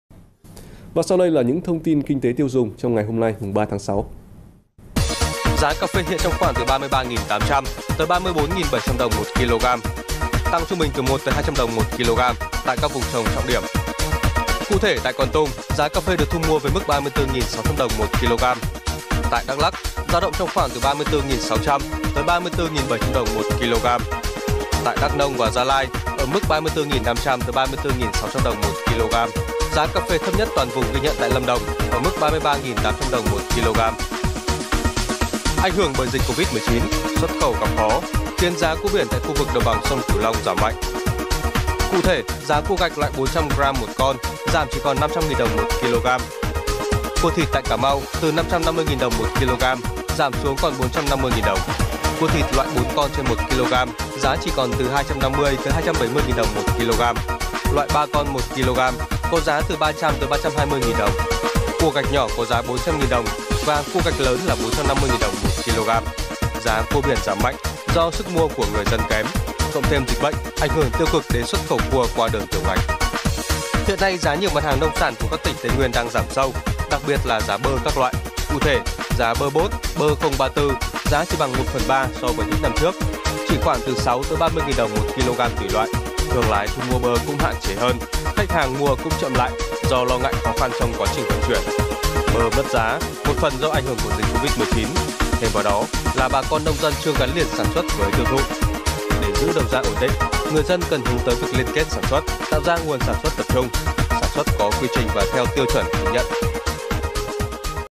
Chùm tin kinh tế tiêu dùng ngày 3/6: Cà phê tăng giá, bơ, cua biển giảm mạnh - VNEWS
VNEWS - Chùm tin kinh tế tiêu dùng ngày 3/6:
+ Cà phê tăng giá
+ Giá cua biển giảm mạnh
+ Giá bơ giảm sâu